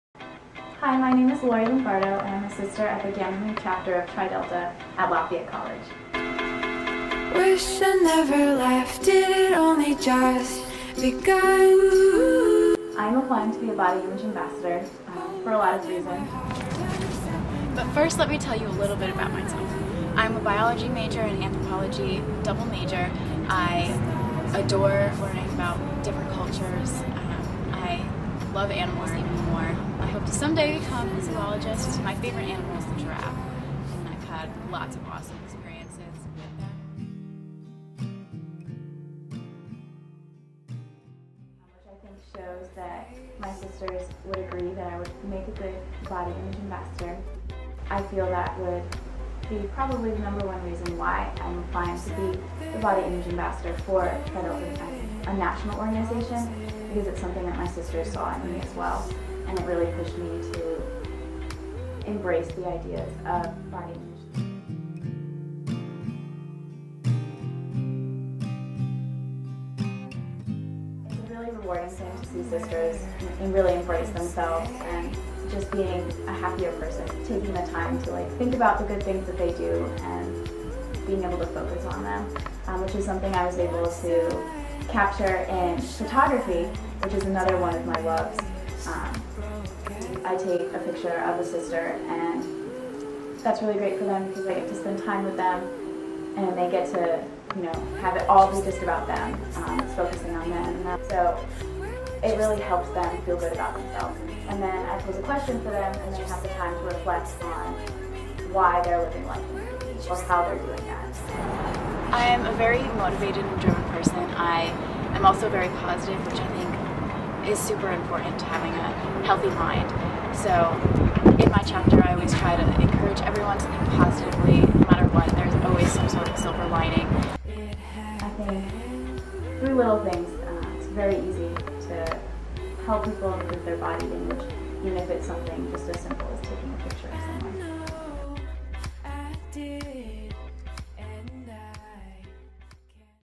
Body Image Ambassador Application
Application video for Tri Delta's National Body Image Ambassador Position.

UPDATE: I received the position!